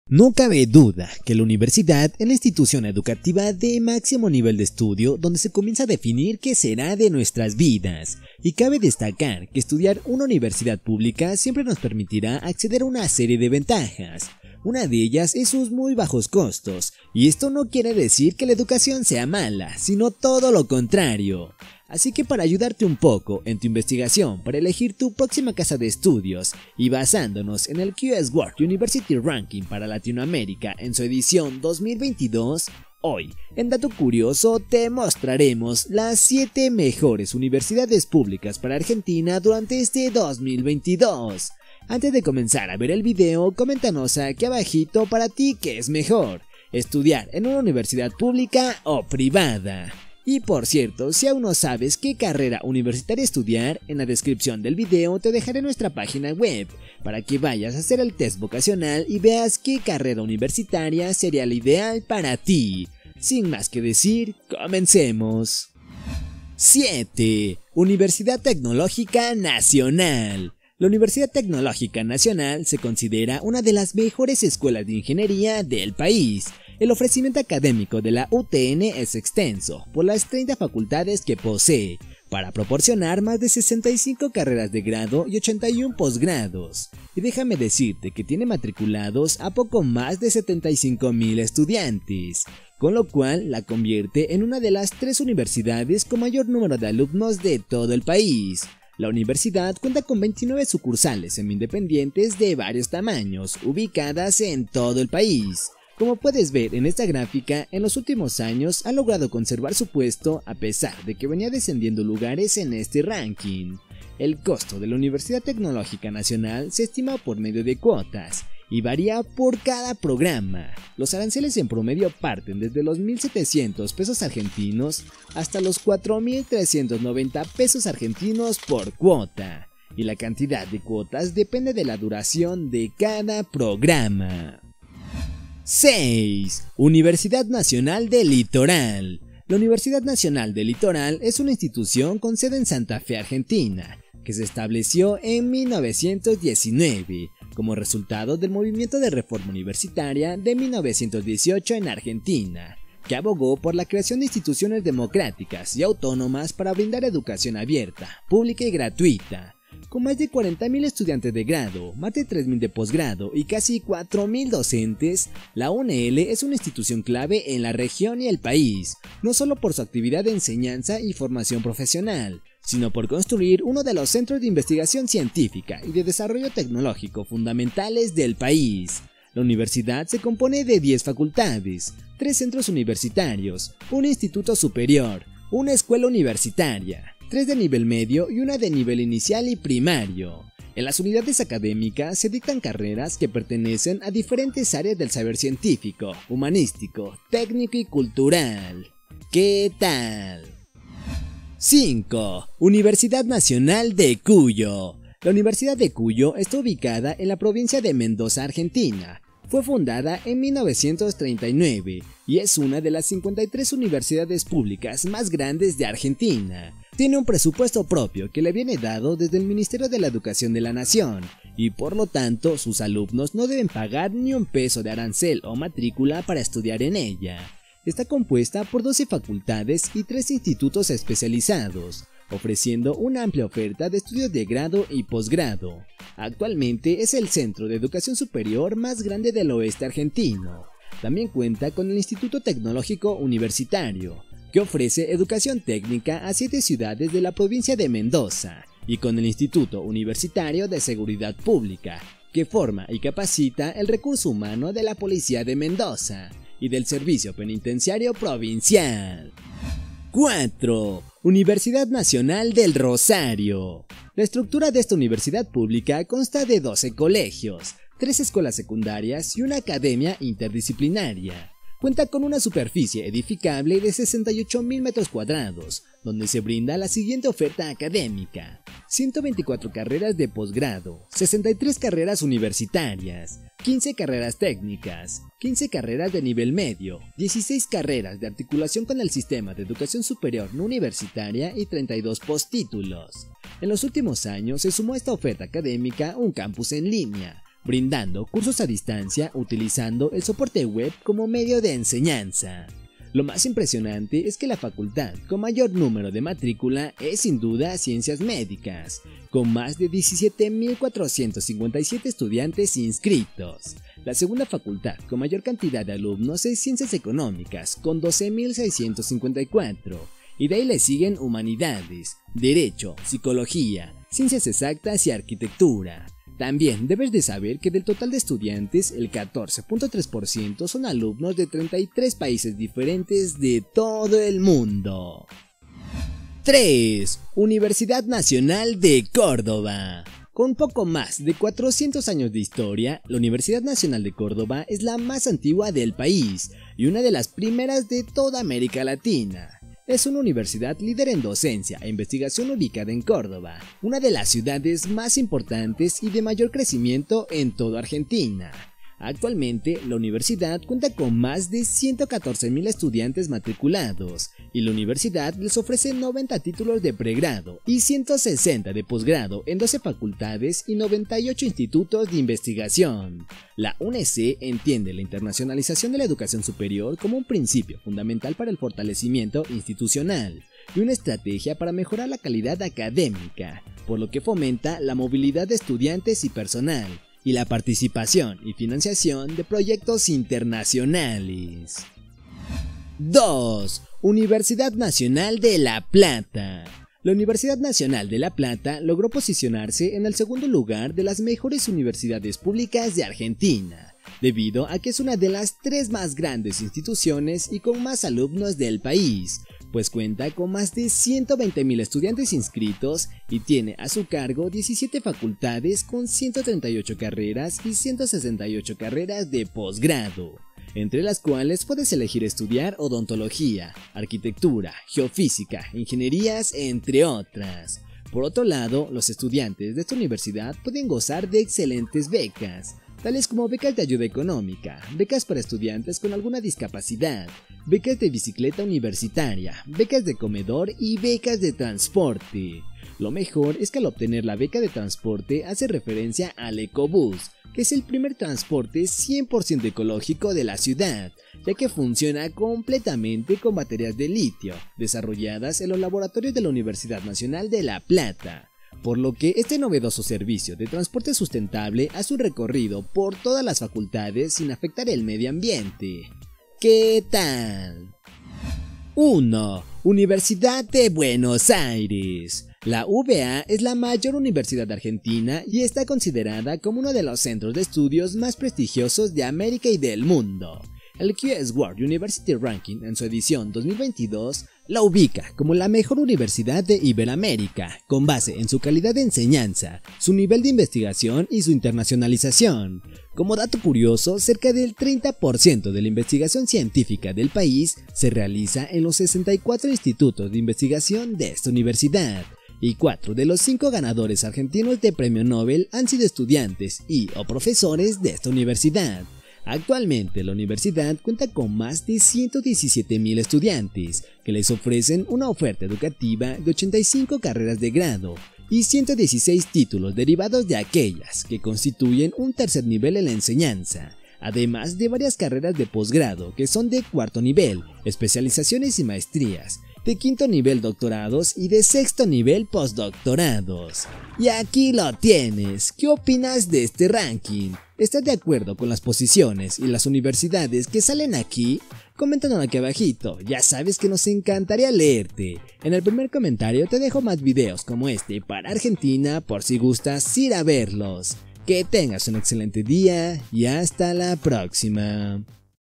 7 Mejores Universidades Públicas Argentina 2025 😎
🔴 7 mejores universidades públicas de argentina 2025

No cabe duda que, la universidad es la institución educativa de máximo nivel de estudio dónde se comienza a definir que será de nuestras vidas. Y cabe destacar que, estudiar en una universidad pública siempre nos permitirá acceder a una serie de ventajas, una de ellas es sus muy bajos costos, y esto no quiere decir que la educación sea mala sino todo lo contrario.
Así que, para ayudarte un poco en tu investigación para elegir tu próxima casa de estudios y basándonos en el QS World University Ranking para Latinoamérica en su edición 2025, hoy en dato  curioso te mostraremos las 7 mejores universidades públicas para argentina 2025, antes de comenzar a ver el vídeo, coméntanos aquí abajito para ti que es mejor ¿Estudiar en una universidad pública o privada? y por cierto, si aún no sabes que carrera universitaria estudiar, en la descripción del vídeo te dejare nuestra página web para que vayas a hacer el test vocacional y veas que carrera universitaria sería la ideal para ti, sin más que decir ¡comencemos!